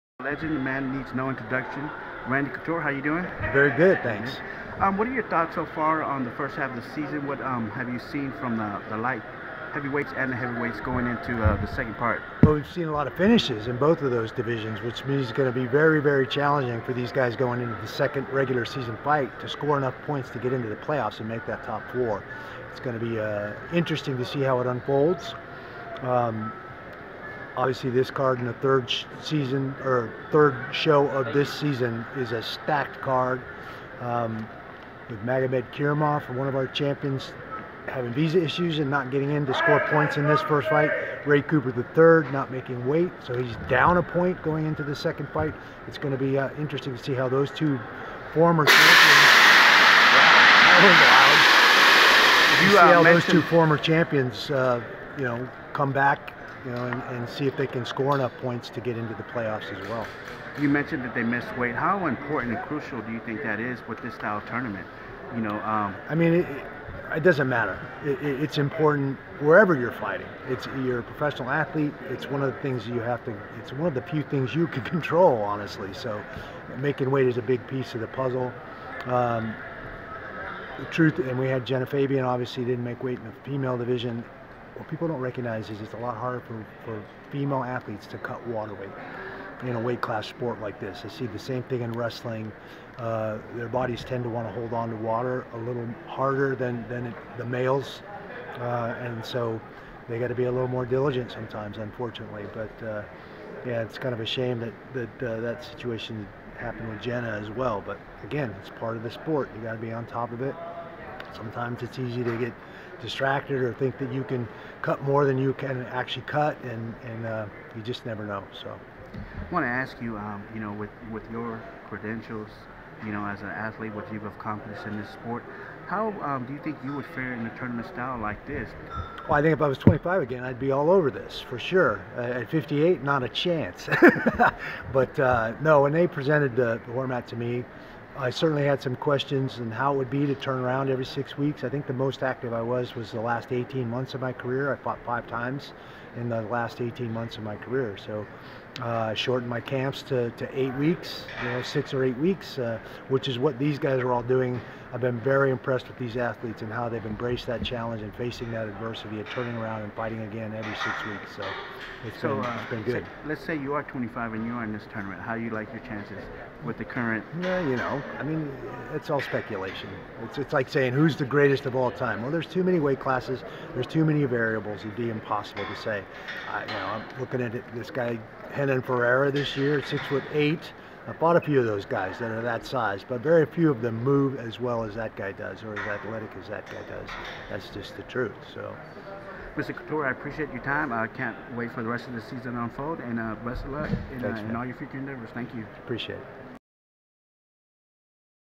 RANDY "THE NATURAL" COUTURE PFL 3 INTERVIEW
PFL 3 2022
Friday May 6. 2022
Arlington, TX

@texasmmaroundup4705 caught up with the legend Randy Couture and asked his thoughts of the upcoming 2022 PFL season and how would younger version of himself fare in this style tournament.